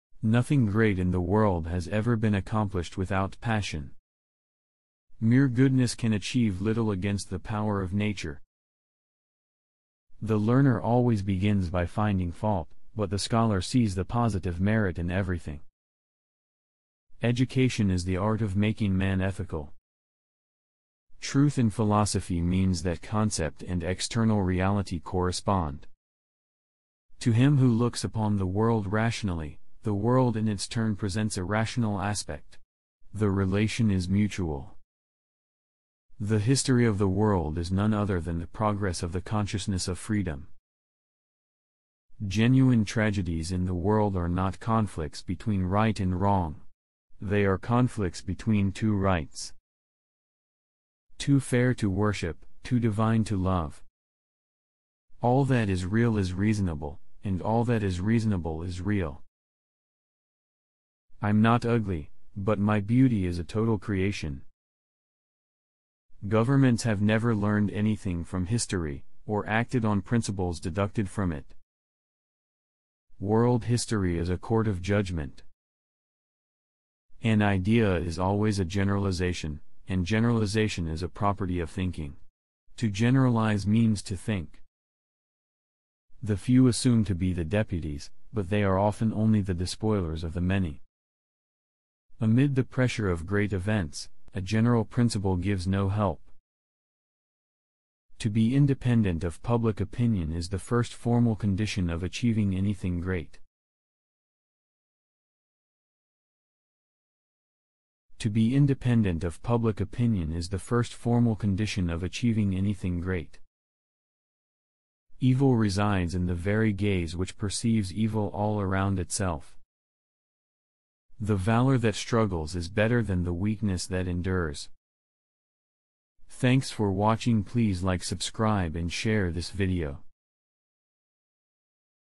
20 Powerful Quotes From Georg Wilhelm Friedrich Hegel You Should Know Before You Get Old
In this video, I'm going to show you a few of the quotes by Georg Wilhelm Friedrich Hegel. He was a German philosopher who developed what we now know as the Hegelian dialectic. The following quotes are from his famous work, "Lectures on the History of Philosophy."

Hegel quotes on understanding, science, philosophy, and history.